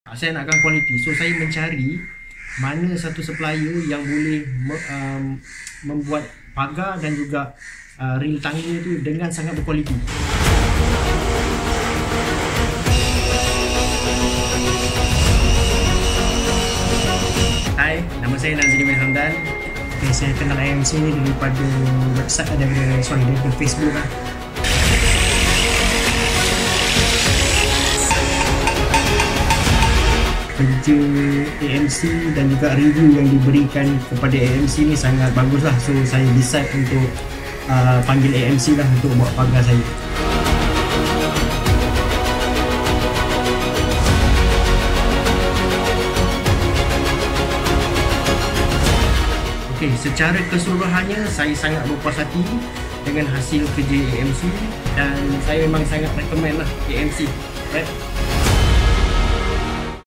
Sebab kenapa En Nazri rekomen AMC kepada anda
Maklum balas pelanggan dengan hasil kerja AMC.

Jom dengar perkongsian En Nazri (pelanggan) ni.

Mesti ada yang kenal siapa En Nazri ni kan?
beliau merupakan salah seorang Tiktoker yang terkenal juga. Terima kasih untuk En Nazri sudi temubual oleh AMC dan share pengalaman sebelum mengenali AMC.

Sila lawat page kami di Facebook :
AMC Engineering Steel Sdn Bhd